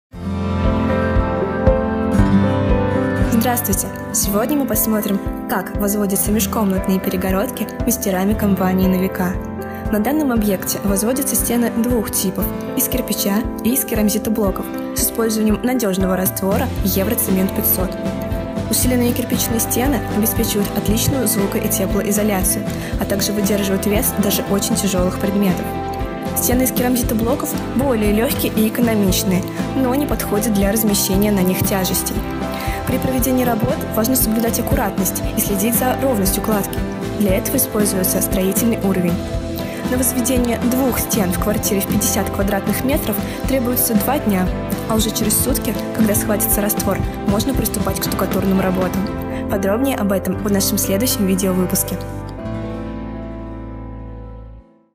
Возведение межкомнатных перегородок из кирпича и керамзитоблоков | На Века
Возведение перегородок компанией "На века".
Наши мастера возводят межкомнатные стены из различного материала: от кирпича до газобетона, всего за 2 дня. В этом видеообзоре показаны работы по возведению перегородок на объекте, где перед этим провели демонтаж старых неровных стен.